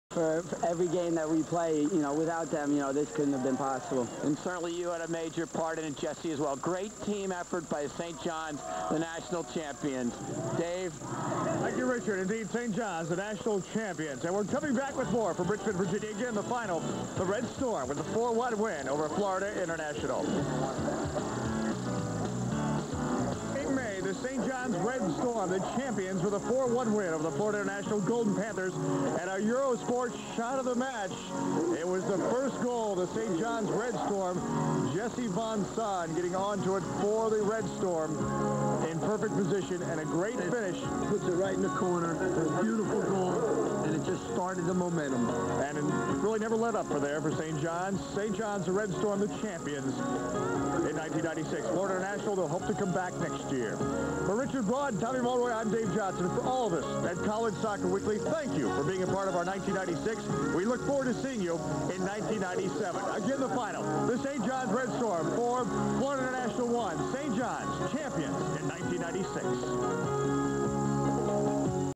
St John's Wins the 1996 NCAA Soccer National Championship
The St. John's Red Storm defeat FIU in the 1996 NCAA Soccer National Championship.